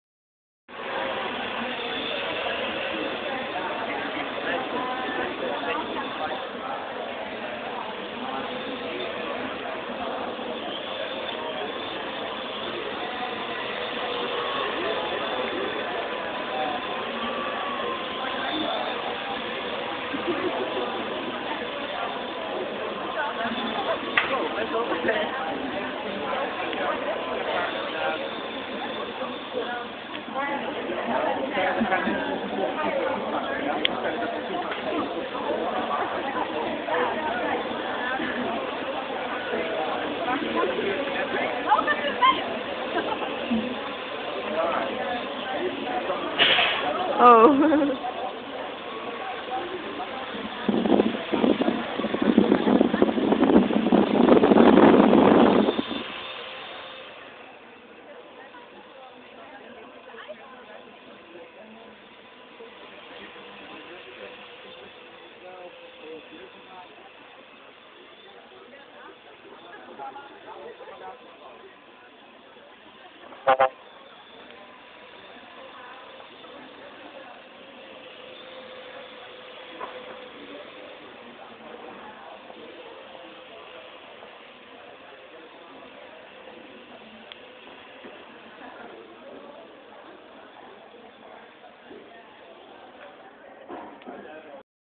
Flying the AR.Drone
Een van de vluchten van de AR.Drone door het Atrium, Oudemanhuispoort, tijdens de Bacheloropendag.